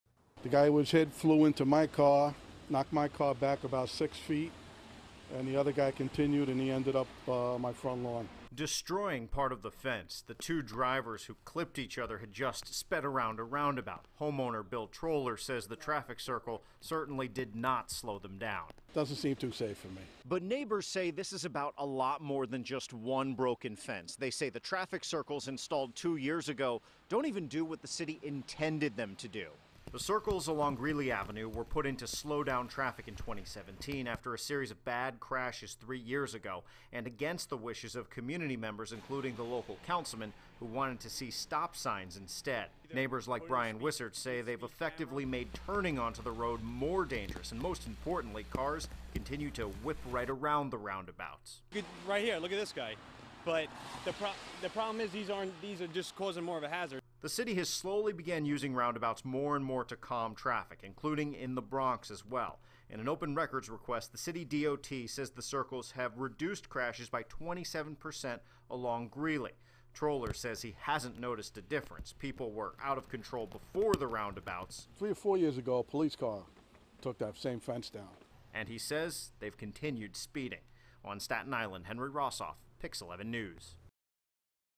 Staten Island residents still frustrated by controversial roundabouts
Residents said traffic circles the city added to slow down speeding cars in their neighborhood are not working and now they have video to prove it.The circles along Greeley Avenue were put in to slow traffic down in 2017 after a series of bad crashes three years ago. It was done against the wishes of many community members, including the local councilman, who wanted to see stop signs instead.